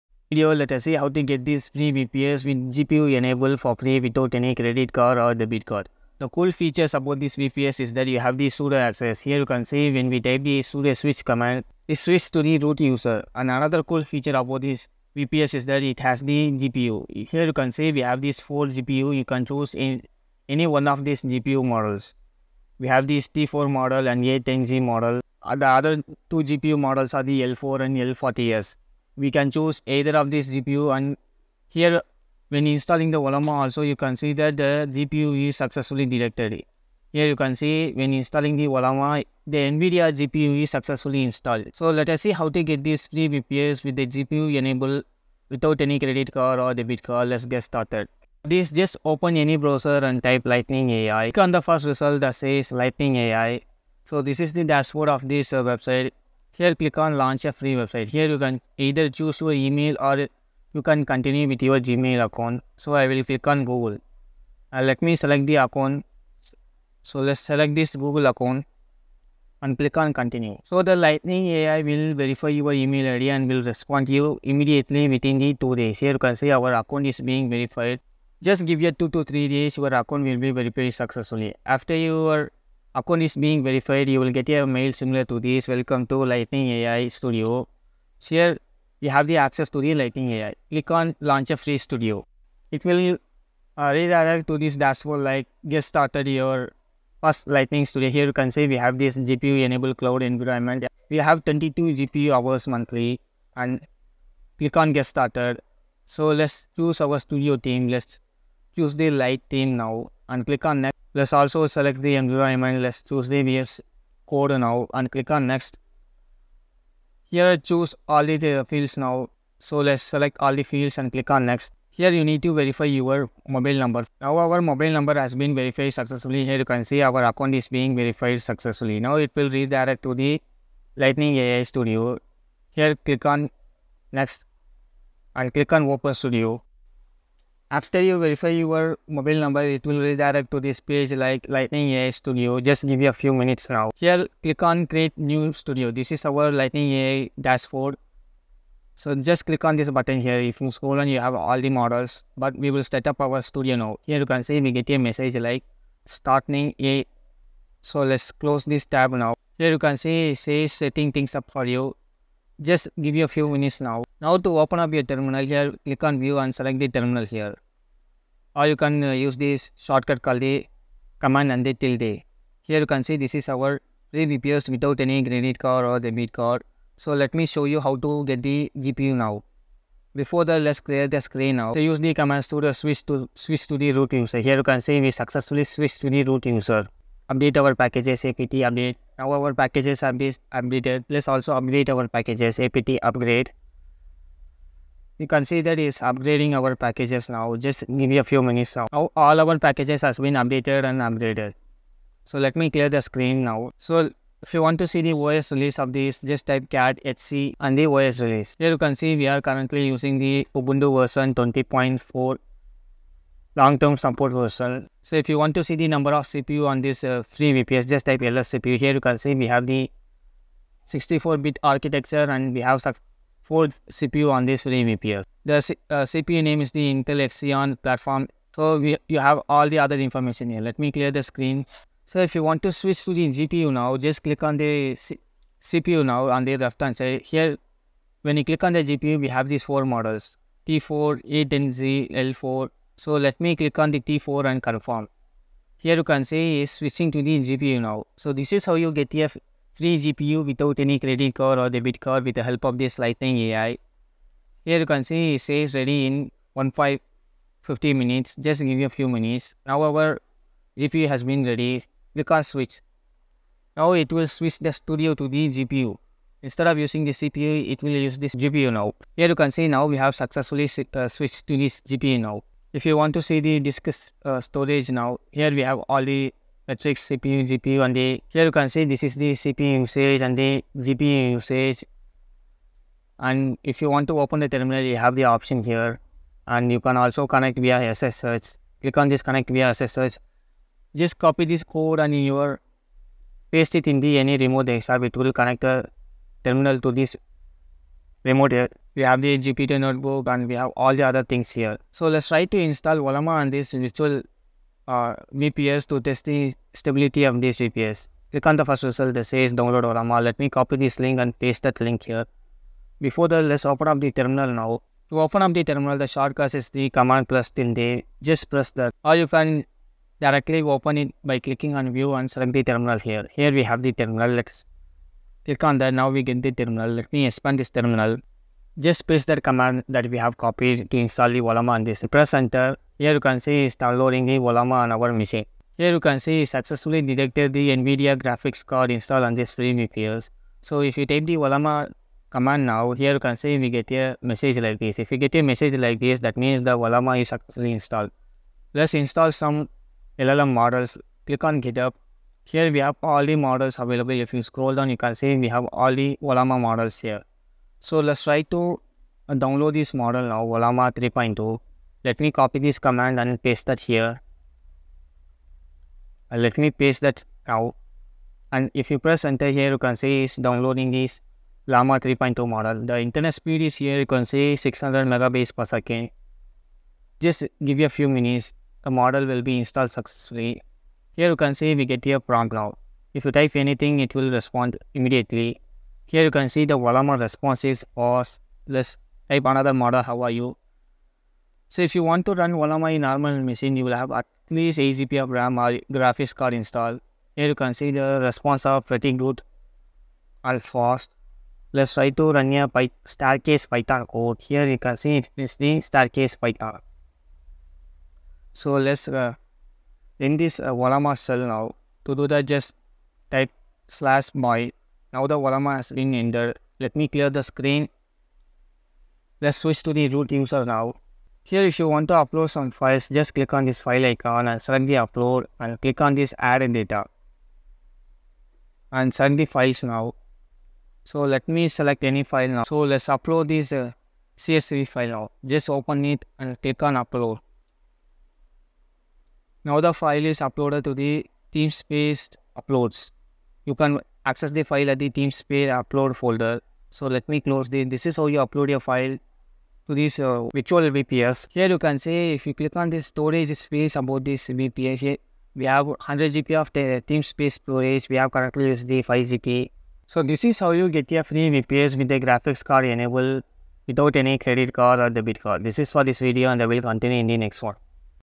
How to Create a FREE VPS 16GB RAM + GPU - Lifetime,SSH Access,100GB Storage | No Credit Card
Looking for a powerful VPS solution without the hassle of credit cards? In this video, I’ll show you how to get a 4-Core VPS with 16GB RAM and GPU access—all for FREE and for LIFE! Perfect for developers, gamers, or anyone needing serious computing power. 💻✨

🔑 What You'll Learn:
💡 Step-by-step guide to getting your VPS
🛠️ Lifetime access with SSH
📦 100GB storage included
🚫 No credit card required!
🌟 Why Choose This VPS?
🔄 High performance for gaming and development
🔒 Secure SSH access for peace of mind
⚡ Fast setup to get you started instantly

Custom VPS solutions
VPS features
Buy VPS online